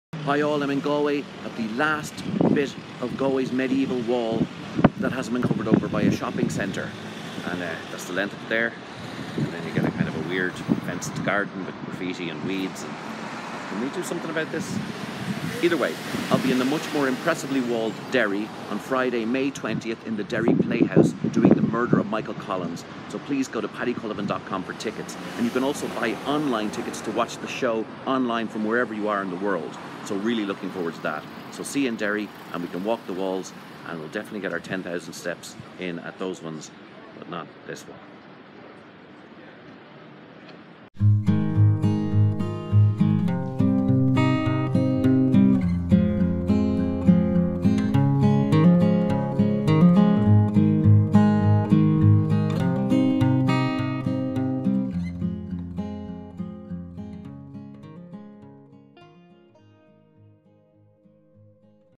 The Murder of Michael Collins | Paddy Cullivan | The Playhouse
Who really shot Michael Collins? How can one of the most important men in Irish history have no autopsy report, no investigation, inquest or even a death cert? Why is there such reticence from certain quarters - to this day - to find out the truth?

Historical Entertainer Paddy Cullivan brings you the incredible story of the death of Ireland’s first Commander-in-Chief. An audio-visual spectacular featuring hundreds of images, shocking new research and incredible songs, Paddy dares to unravel the secrets and lies around what happened that fateful day in Béal na mBlath, 22nd August 1922.

"MESMERISING STUFF - A COMPLICATED STORY, BRILLIANTLY TOLD"
★★★★★ IRISH TIMES

Performed in theatre and broadcast live on 20th May at 8pm.